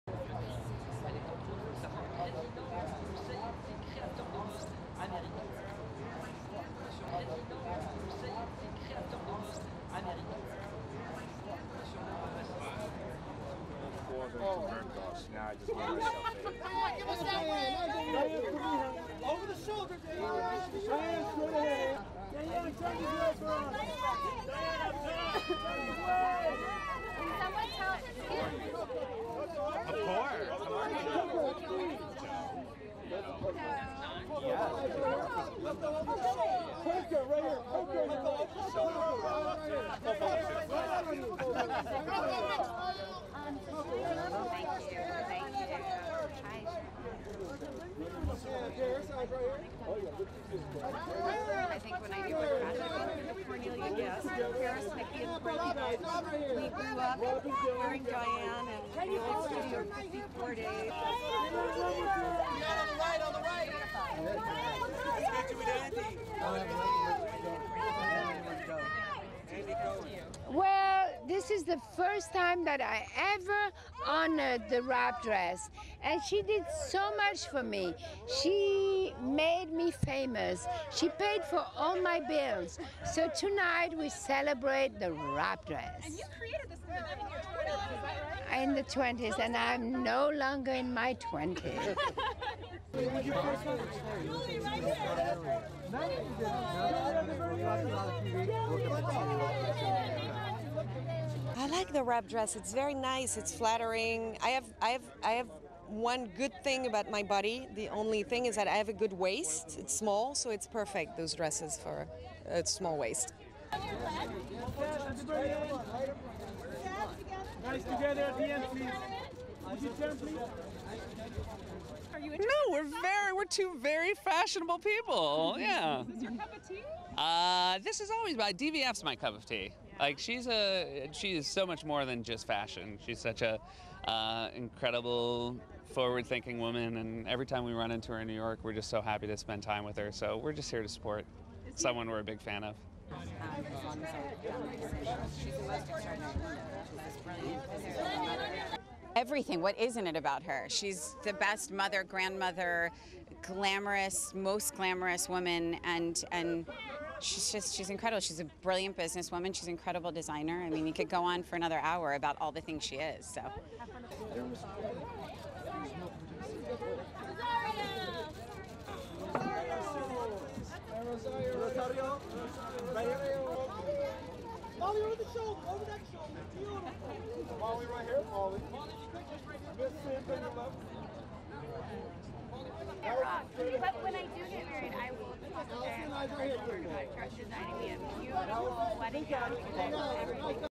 Stars celebrate the 40th anniversary of Diane von Furstenberg's iconic wrap dress
(11 Jan 2014) 40TH ANNIVERSARY OF DIANE VON FURSTENBERG'S WRAP DRESS CELEBRATED WITH EXHIBIT, STARRY PARTY
It takes a pretty special dress to merit its own star-studded anniversary party.
The wrap dress, designed 40 years ago by Diane Von Furstenberg, was the guest of honor Friday (10 Jan.) at the opening of the designer's "Journey of a Dress" exhibit in Los Angeles.
"Well this is the first time that I ever honored the wrap dress and she did so much for me. She made me famous, she paid for all my bills. So tonight we celebrate the wrap dress," said the Belgian born fashion designer before the gala.
Marisa Tomei, Paris Hilton, Rosario Dawson, Robin Wright and Julie Delpy were among the many stars celebrating Von Furstenberg's enduring design.
"I like the wrap dress. It's very nice. It's flattering," said Delpy. "I have one good thing about my body, the only thing is that I have a good waste. It's small so it's perfect those dresses for a small waist."
Fastened with a simple tie at the waist, the easy-to-wear style has remained unchanged for generations.
"She is the best mother, grandmother, glamorous, most glamorous woman and she's incredible. She's a brilliant businesswoman. She's an incredible designer. I mean I could go on for another hour about all the things she is," gushed designer Rachel Zoe.
Comedian Seth Meyers also attended the soiree with his wife, Alexi Ashe.
"DVF is my cup of tea," said Meyers. "Like she is so much more than just fashion. She's such an incredible forward thinking woman and every time we run into her in New York we are just so happy to spend time with her. So we are just here to support someone we are a big fan of."
On view until April 1, the exhibit features the designer's favorite dresses from throughout the decades, along with new creations based on vintage styles and fabrics. There are also paintings and portraits of Von Furstenberg from artists such as Andy Warhol and Annie Leibovitz.